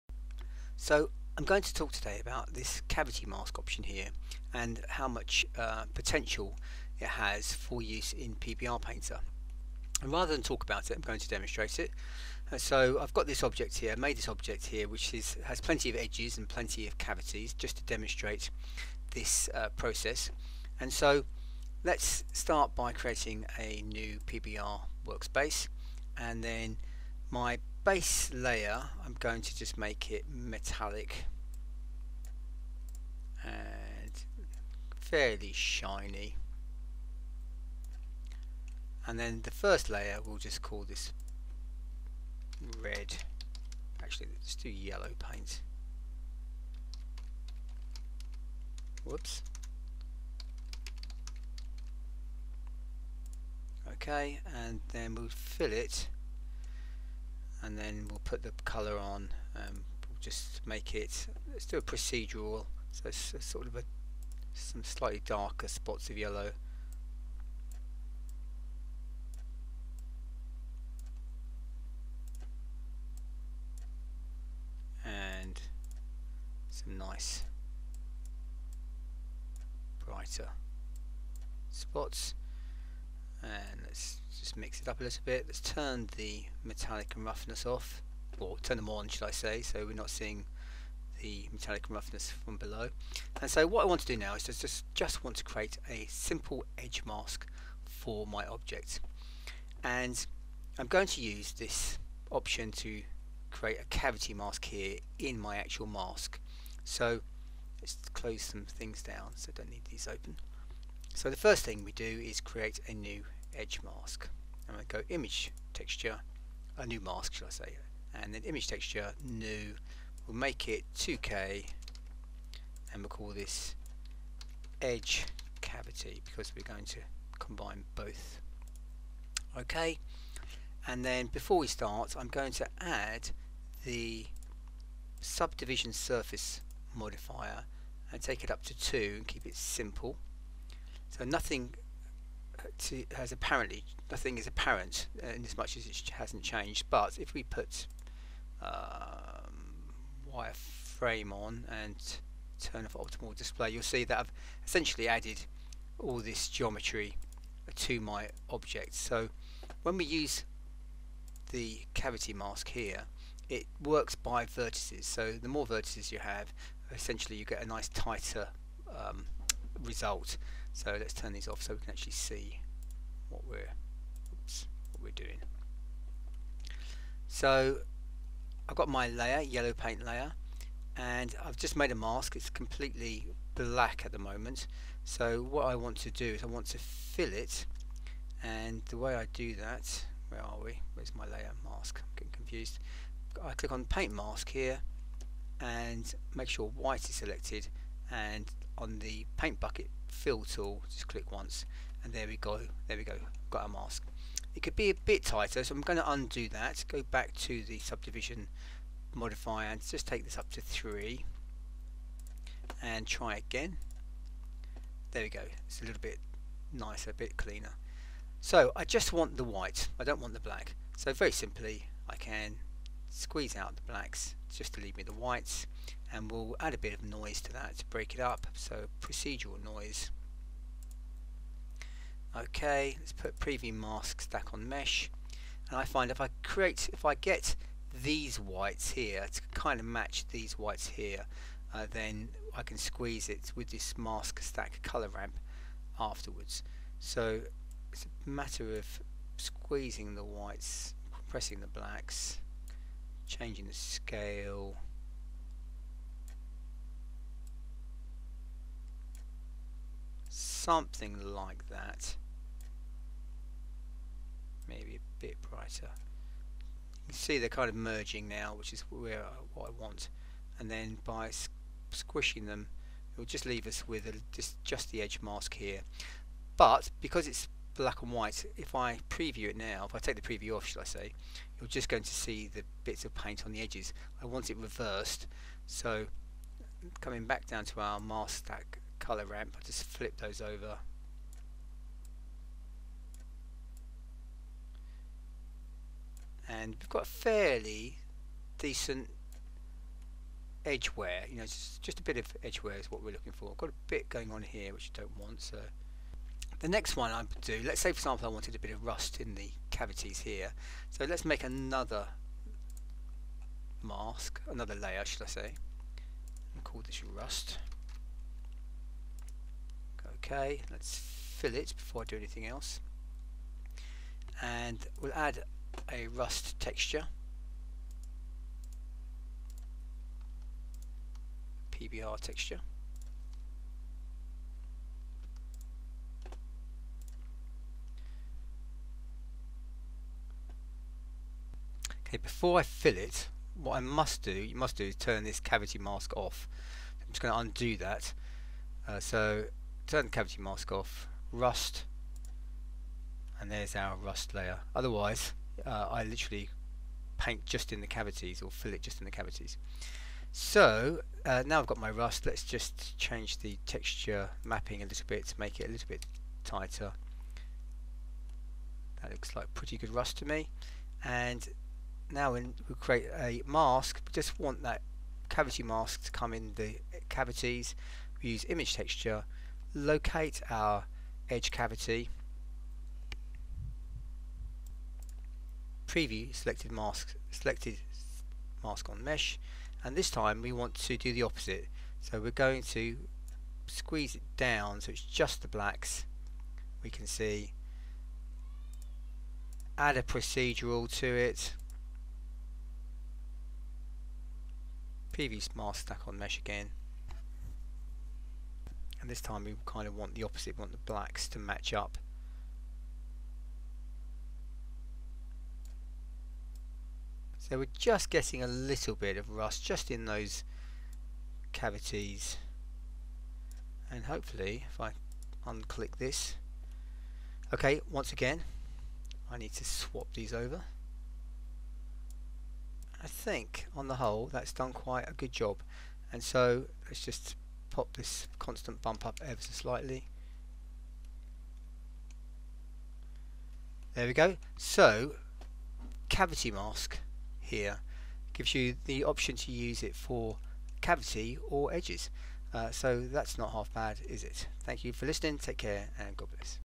Cavity or Edge Mask for PBR Painter in Blender 2.9
Sometimes it can be difficult to create either just an edge mask or just a cavity mask for use in PBR Painter. Using the "Cavity Mask" option in the Masking section of the Brush setting allows you (in certain circumstances) to create a mask that cover both edges and Cavities.